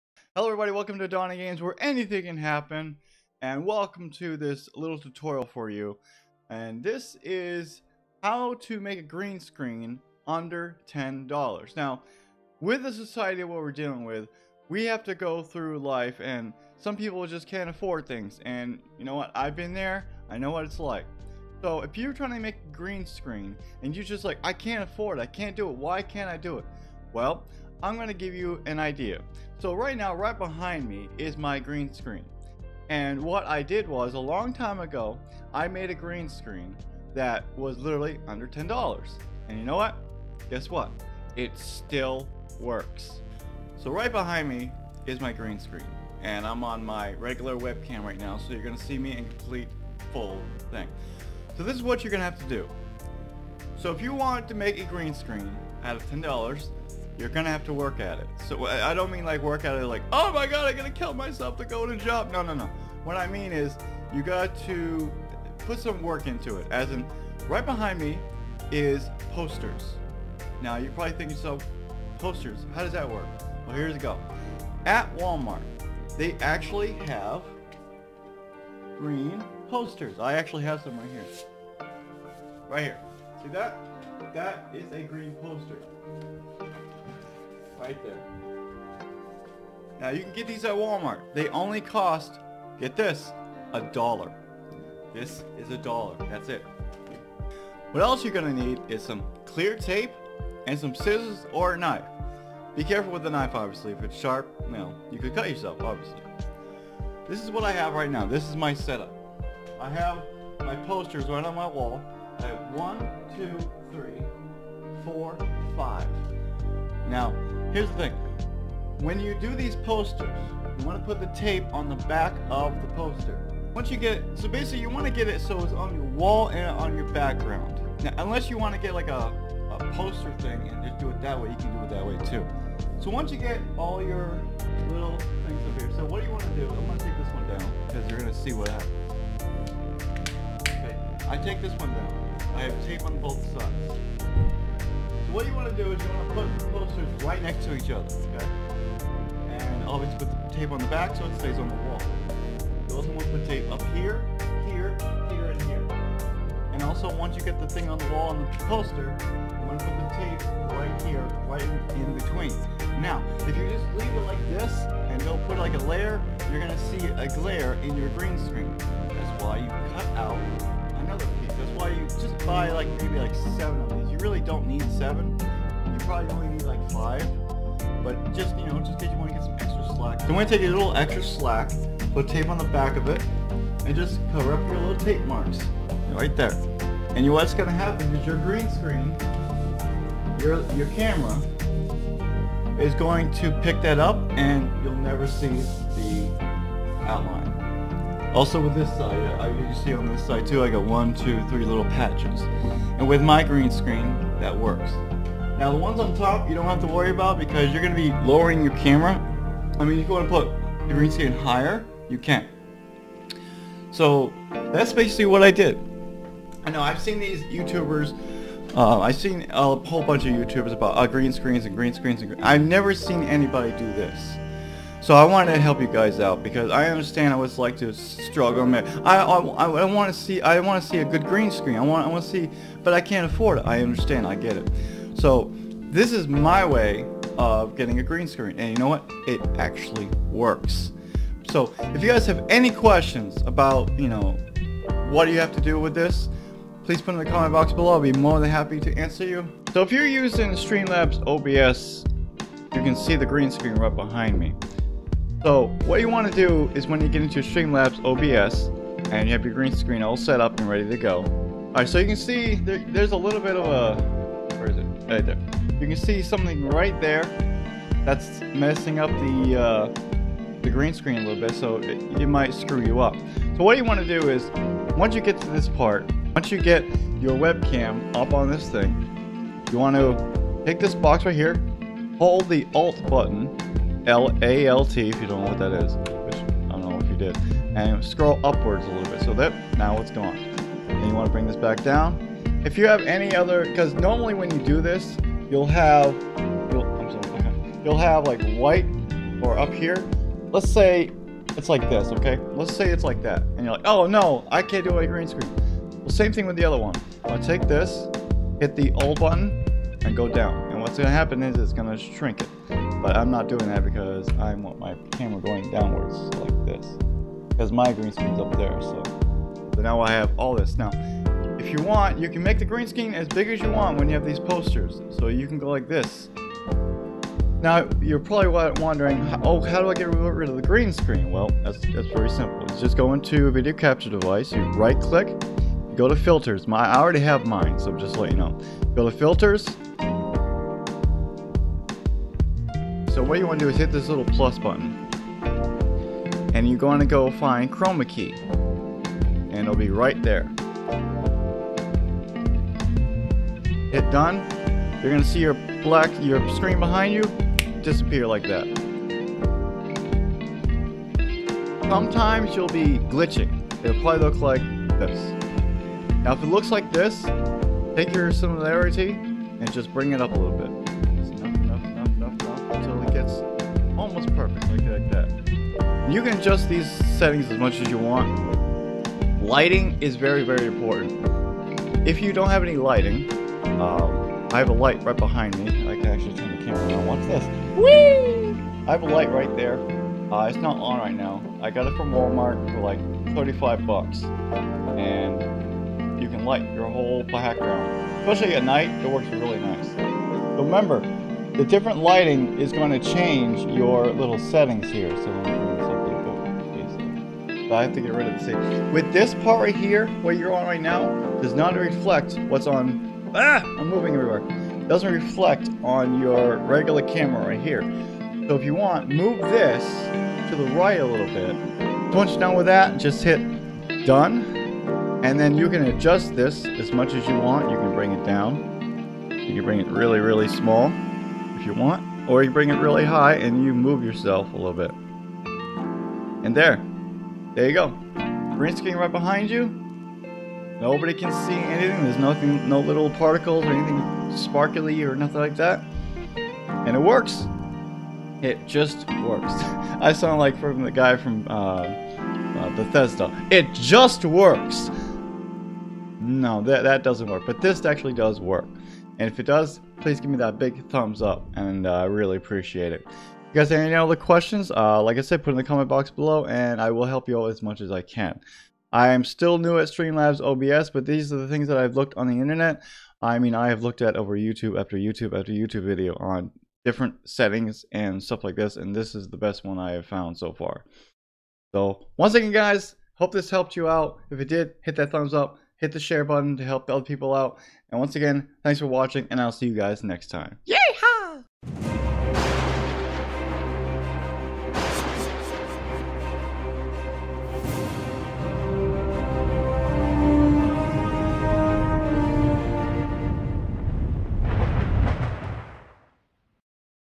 How To Make A Green Screen Under $10.00 Dollars and Use it on Streamlabs OBS
In this how to tutorial video, I show you How To Make A Green Screen Under $10.00 Dollars and Use it on Streamlabs OBS

Intro is From Introchamp.com

Love Streamlabs OBS?